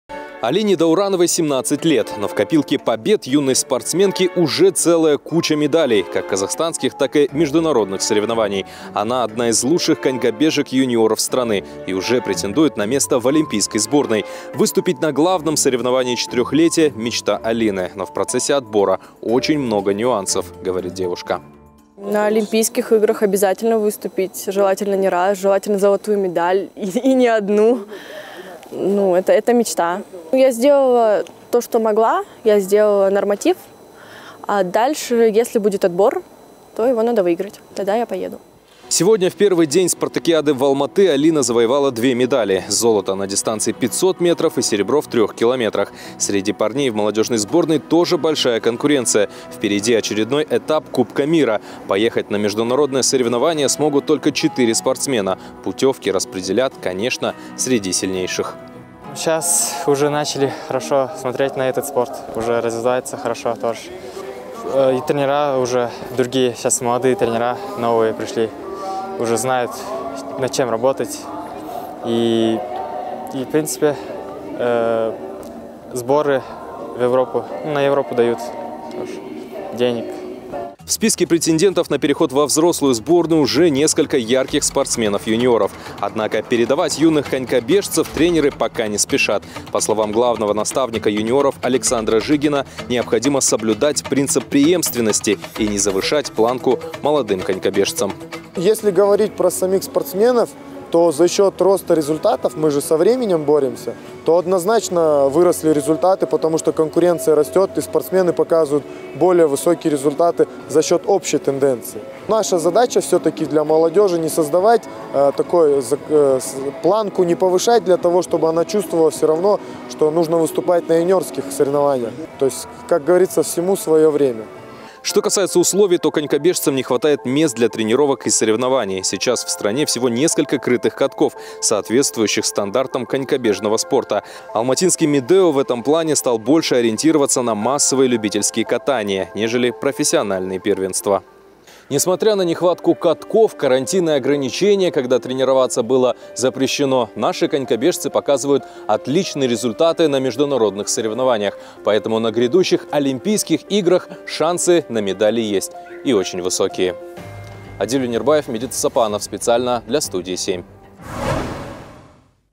Быть на коньке: у Казахстана 7 медалей Чемпионата четырёх континентов
Сборная по конькобежному спорту завоевала в Канаде на чемпионате четырёх континентов семь медалей - три золота, два серебра и две бронзы.
Адиль Онербаев расскажет, как готовятся к серьёзным стартам молодые представители этого вида спорта, и чего ждать от нашей сборной в финале чемпионата.

Во всём информационном потоке мы выбираем то, о чём действительно стоит задуматься. С нами Всё станет понятно